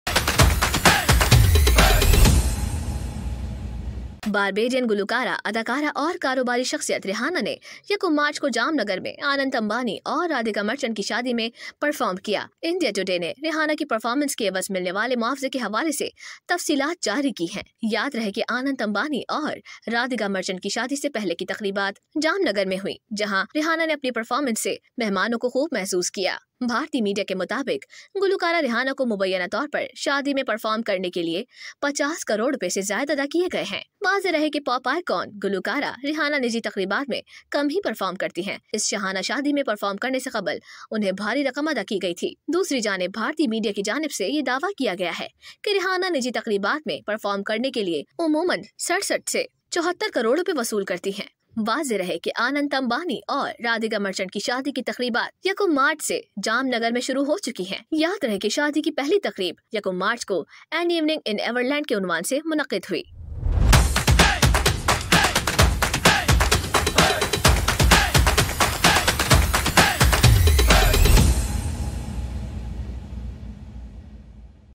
Rihanna's Mind-Blowing Paycheque for Anant Ambani's Pre-Wedding Celebration | Hungama Express
Rihanna's Jaw-Dropping Paycheque for Anant Ambani-Radhika Merchant's Pre-Wedding Party

Prepare to be amazed as Rihanna takes the stage at Anant Ambani and Radhika Merchant's pre-wedding bash in Jamnagar, Gujarat, with a paycheck that will leave you stunned! The R&B sensation is reportedly being paid a staggering £5 million (₹52 crore) for her performance at the star-studded event.

Join us as we delve into the extravagant celebrations, which are estimated to cost the Ambanis a whopping £120 million. From Rihanna's arrival with partner A$AP Rocky to the lavish guest list, including Bollywood A-listers like Shah Rukh Khan, Deepika Padukone, and Ranveer Singh, this event is set to redefine luxury.

Discover the details of the pre-wedding festivities, from the 'anna seva' feeding thousands of villagers to the anticipated 'Evening in Everland' event. With celebrities, billionaires, cricketers, and politicians in attendance, this soirée promises to be the event of the year.